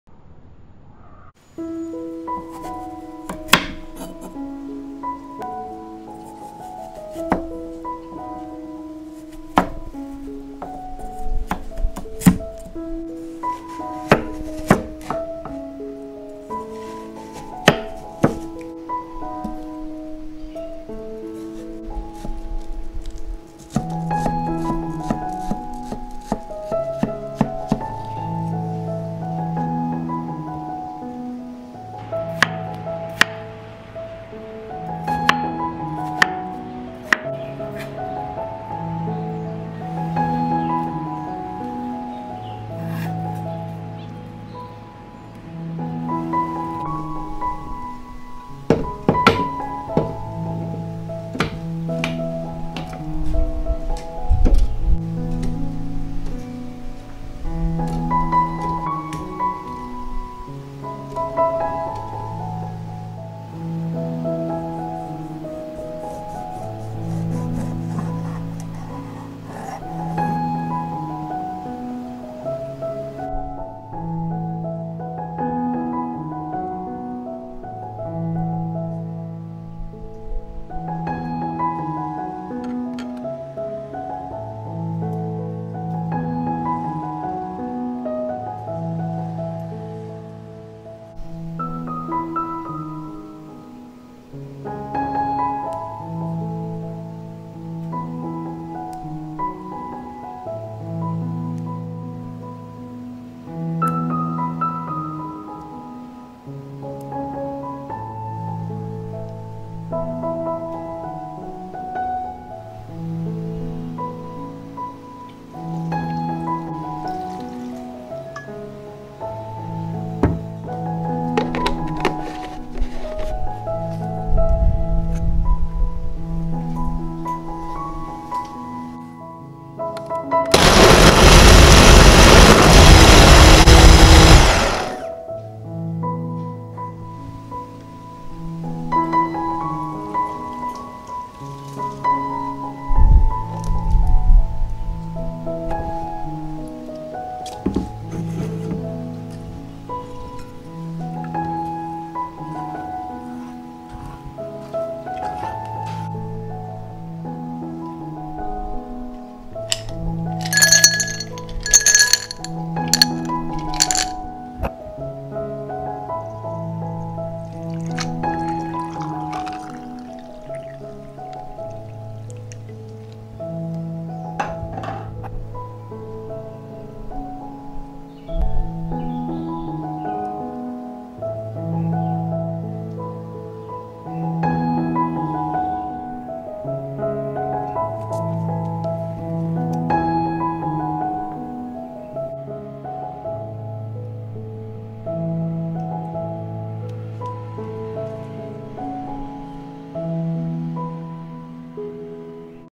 [ASMR] Watermelon Mojito Mocktail
What do you need during a hot day? A refreshing drink! This easy and delicious mocktail only takes 5 minutes to make. I hope you guys will give it a try.

Recipe :
1/4 part of watermelon
12 mint leaves
1 lime, squeeze the water
1 cup of water
Honey or ant other sweetener you like (optional)

Love,
Pomme Story